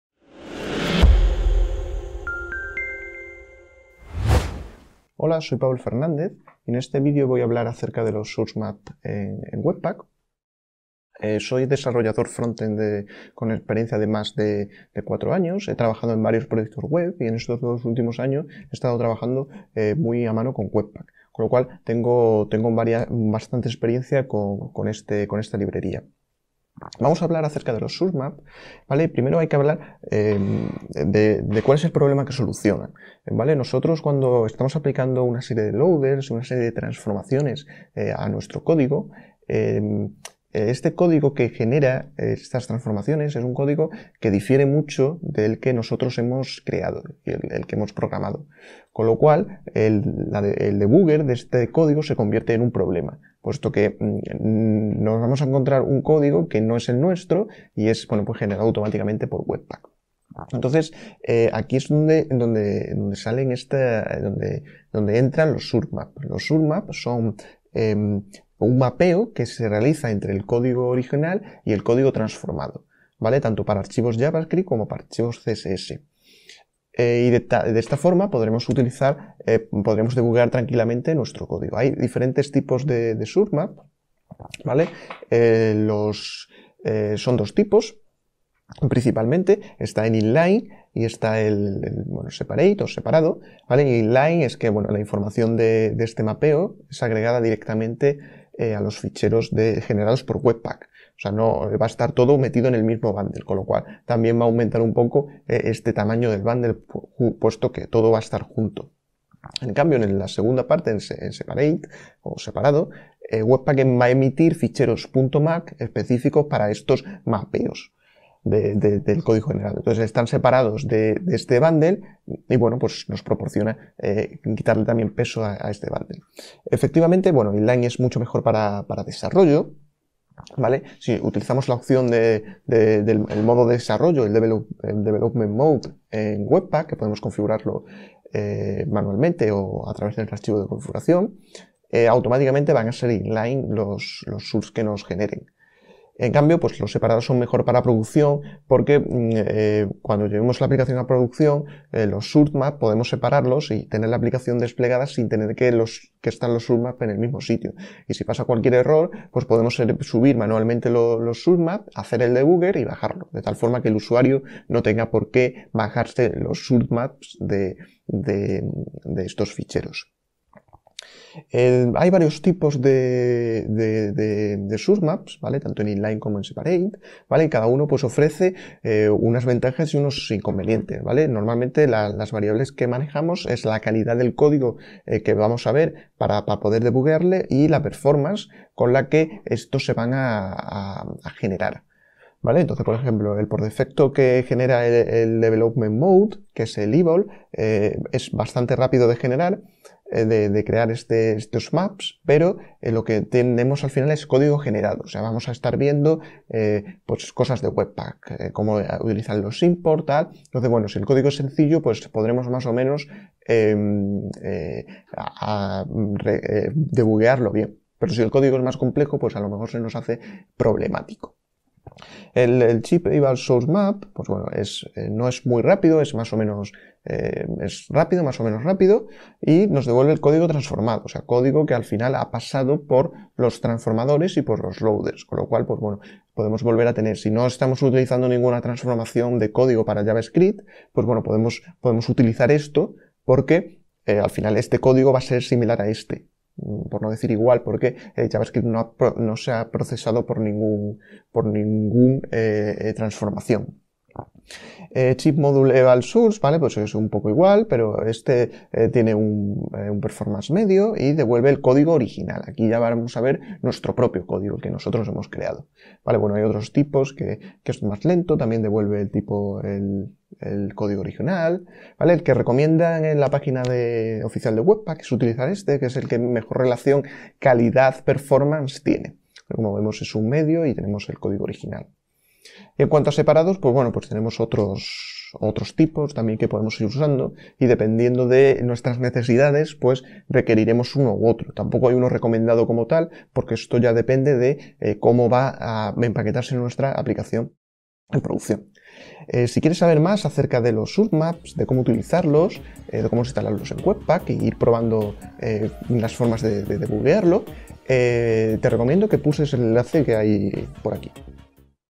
WEBPACK SOURCE MAPS: ¿QUÉ SON?
Te contamos qué son los Source Maps en Webpack, que problemas solucionan y qué tipos podemos utilizar.

INDICE

01# Qué es source maps - 0:26
02# Tipos de source maps - 1:21
03# Diferentes source maps - 2:50